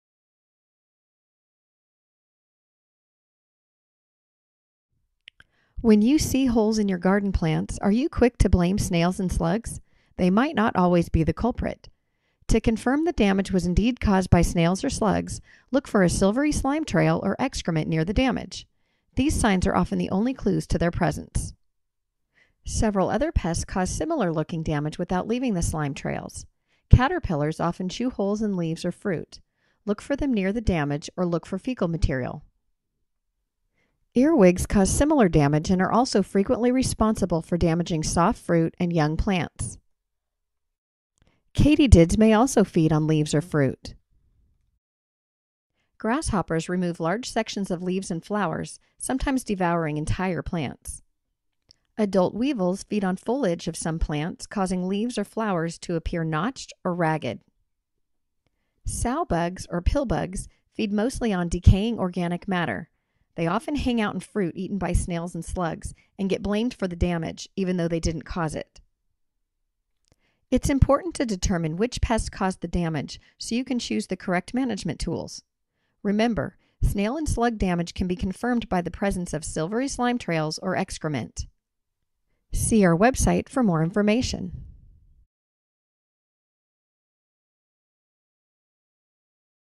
Did a snail eat my plant? - UC IPM
Many pests can chew holes and cause damage similar to snails and slugs.  Learn to recognize the signs to confirm snails ate your plant.